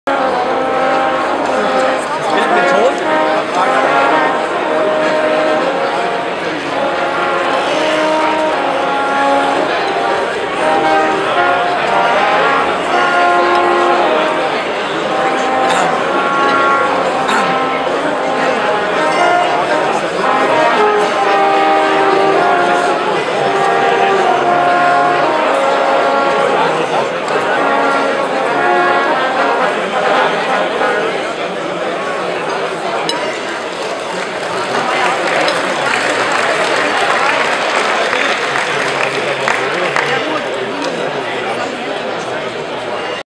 Drechseltreffen 2013 in Seeg Zimmermusi
Ein tolles Rahmenprogramm, kommt leider wegen der hohen Hintergrundlautstaerke nicht so richtig raus - war aber zuenftig